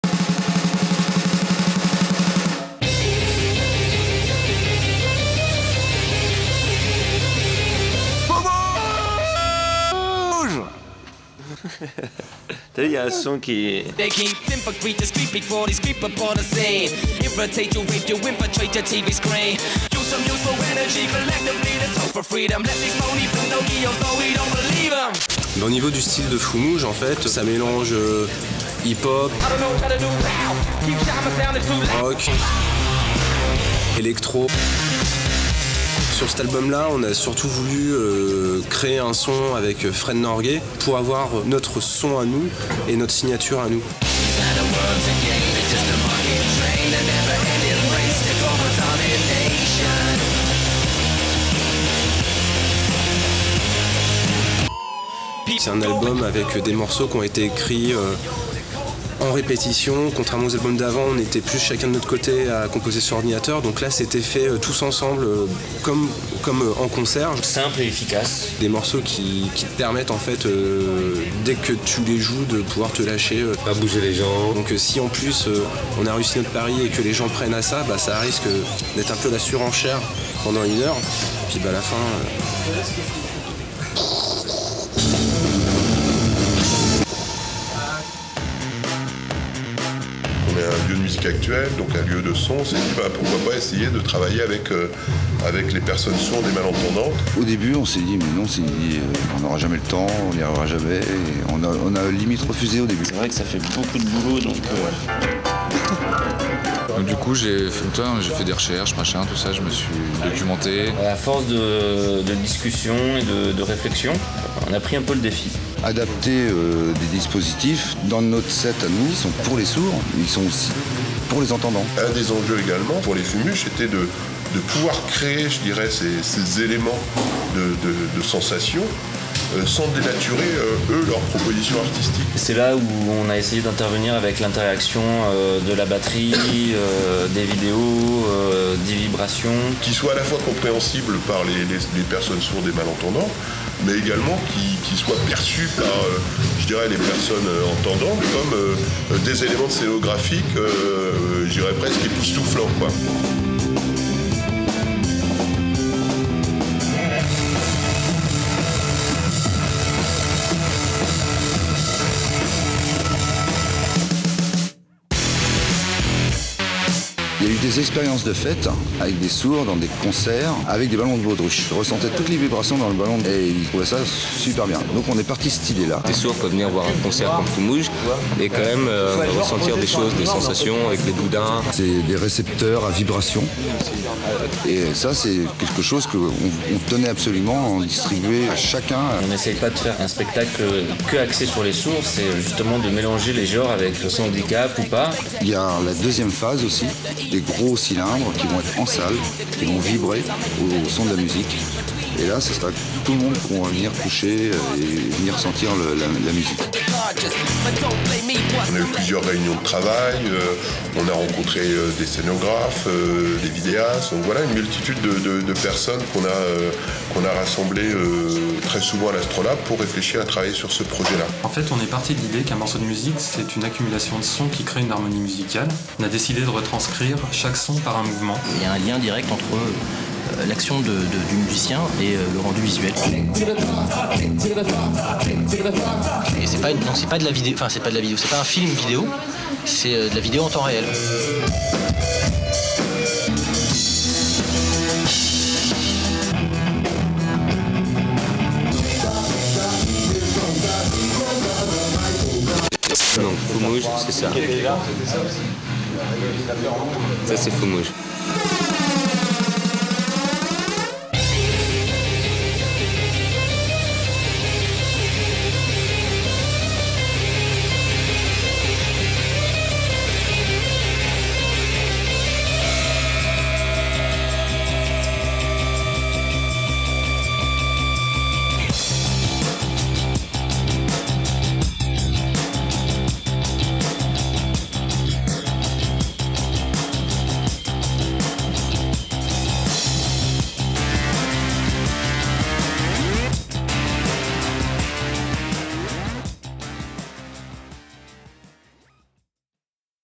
EPK Fumuj - Play or Die Tour
Voici l'EPK de Fumuj, groupe de Tours, qui a préparé à l'Astro son nouveau spectacle "Play or Die Tour". Vous comprendrez à travers la vidéo comment cette tournée s'est montée, et où Fumuj souhaite vous emmener dans un univers où le rock, le hip hop et l'électro deviennent agitateurs des sens au travers de l'énergie savamment distillée sur scène ...